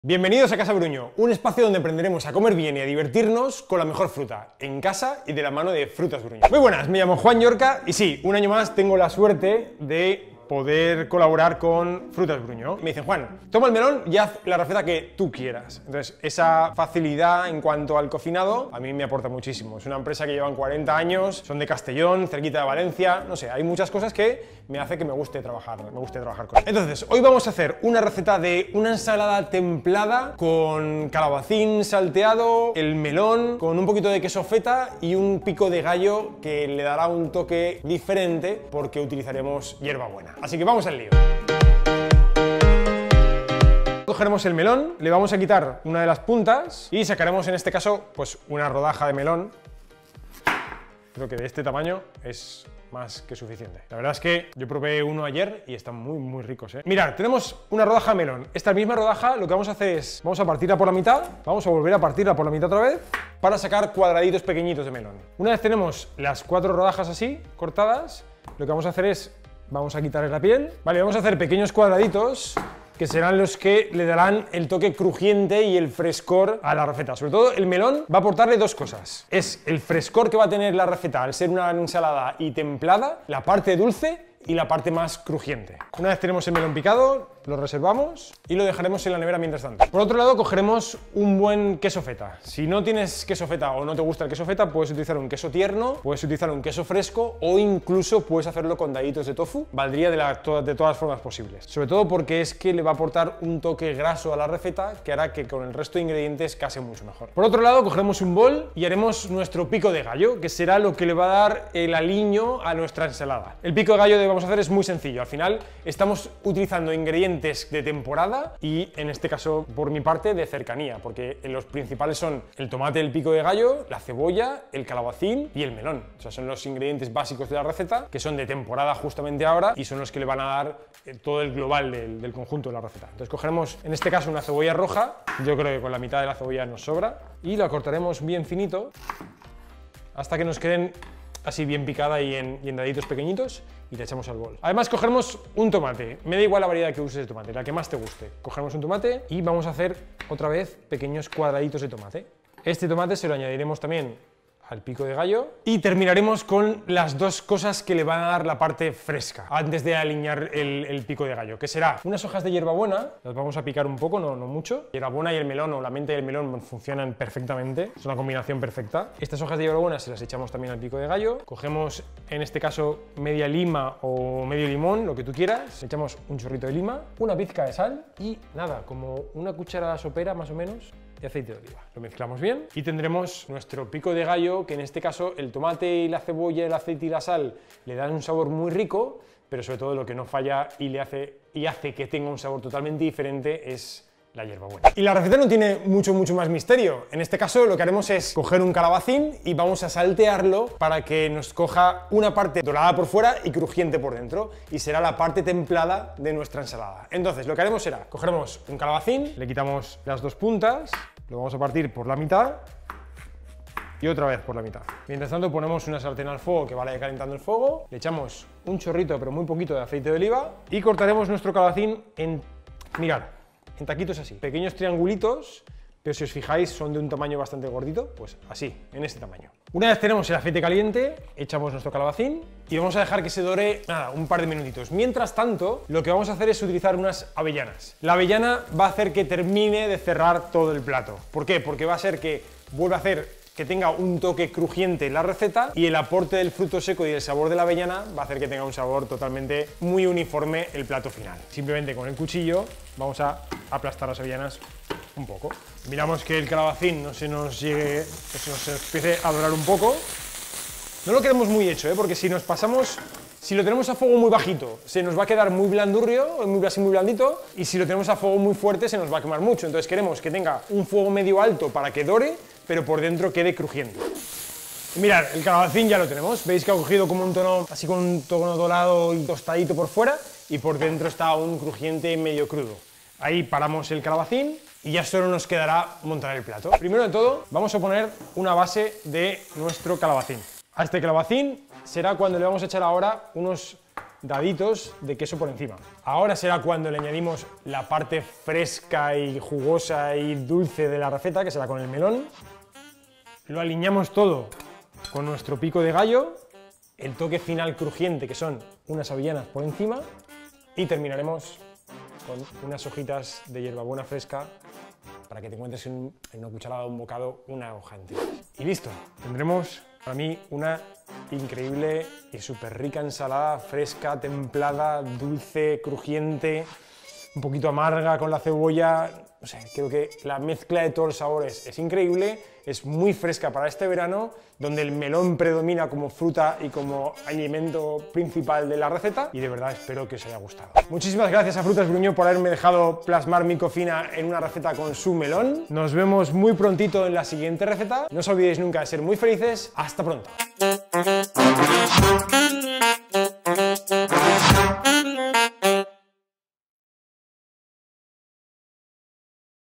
CASA BRUÑÓ CON JUAN LLORCA - Ensalada templada con melón, queso feta y avellanas
¿Cómo hacer un plato sano para los más pequeños y que les encante?

El chef Juan Llorca,  apasionado de la nutrición infantil, visita Casa Bruñó y nos prepara una rica ensalada templada, con daditos de calabacín salteados con melón, queso feta, avellanas y hierbabuena.

El crujiente del calabacín, el frescor y dulzor del melón Bruñó y el toque final que Llorca le da a la receta con unas avellanas, hacen de este plato sencillo una delicia para hacer en familia.

¡Que la disfrutéis!